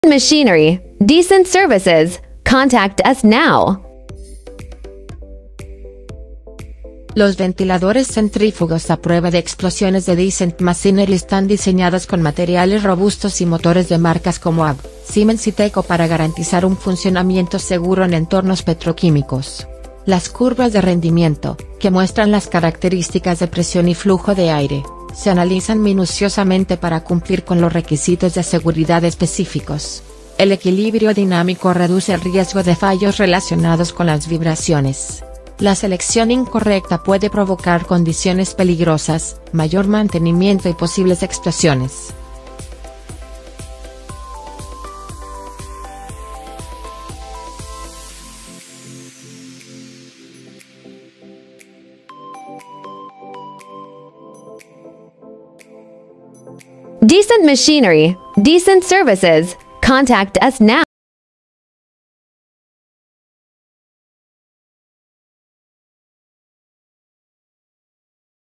¿Cómo garantizan la seguridad los ventiladores centrífugos a prueba de explosiones?
Los ventiladores centrífugos a prueba de explosiones de DECENT MACHINERY están diseñados para garantizar la seguridad en entornos críticos como las plantas petroquímicas. Con tecnología avanzada, estos ventiladores proporcionan una solución confiable para la ventilación en áreas de alto riesgo, minimizando el peligro de explosiones. Para obtener más información, visita nuestro sitio web No olvides ver nuestro video de la fábrica para conocer nuestras instalaciones y procesos de producción